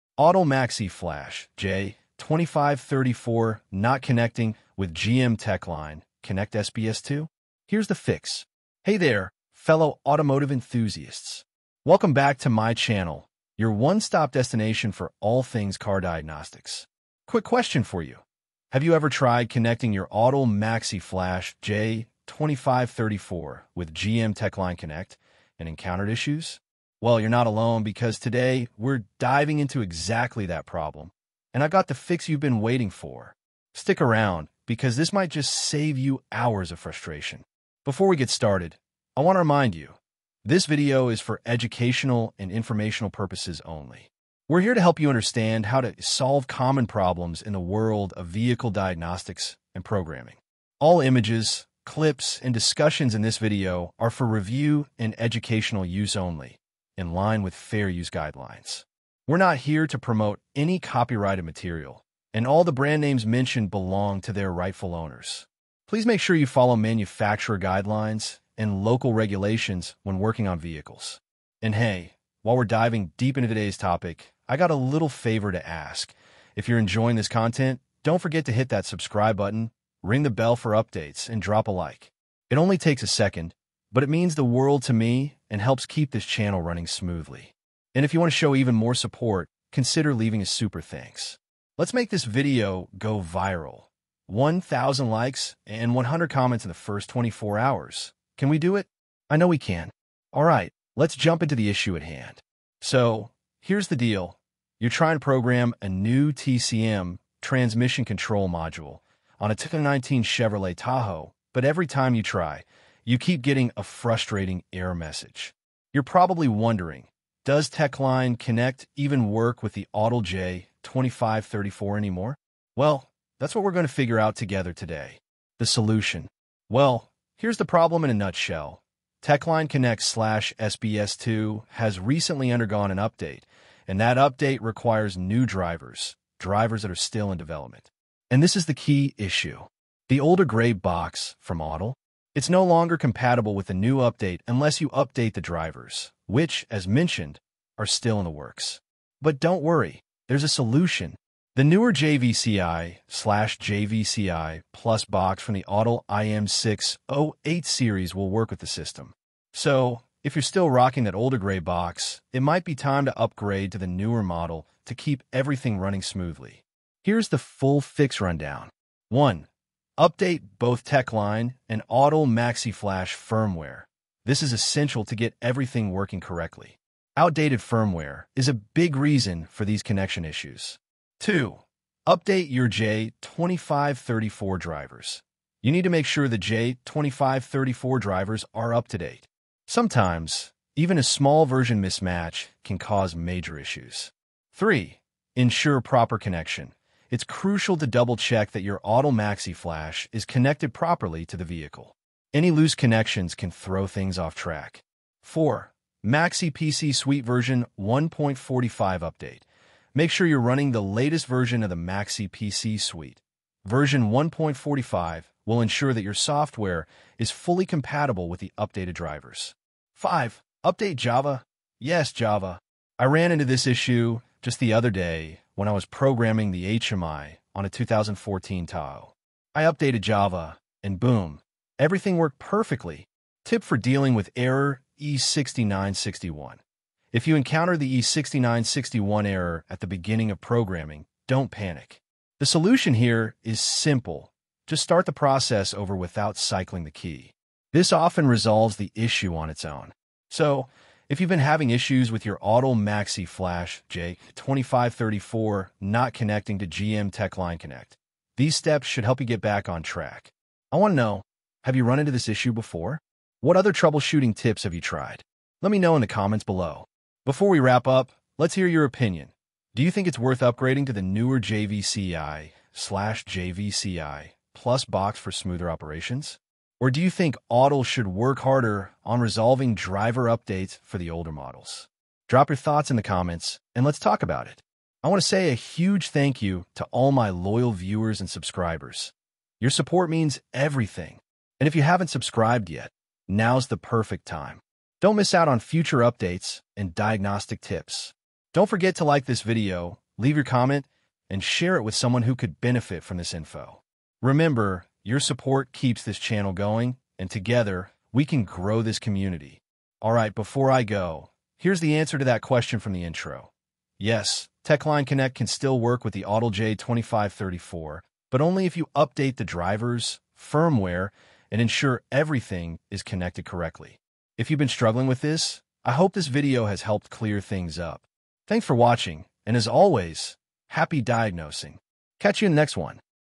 Autel MaxiFlash J2534 Not Connecting with GM Techline Connect SPS2: Here’s the Fix!
Are you facing connection issues between your Autel MaxiFlash J2534 and GM Techline Connect SPS2? You're not alone! In this video, we break down why your Autel J2534 device may not be connecting properly—and more importantly, how to fix it. Whether you're programming a 2019 Chevrolet Tahoe or troubleshooting the infamous E6961 error, this guide will walk you through the exact steps to resolve it.

We’ll cover critical updates including Techline Connect firmware, J2534 driver compatibility, Maxi PC Suite v1.45, and even that sneaky Java update most people forget about. Plus, we’ll explain why upgrading to the Autel JVCI/JVCI+ might be the smart move for seamless GM programming.

💬 Drop your thoughts and experiences in the comments—have you faced this issue?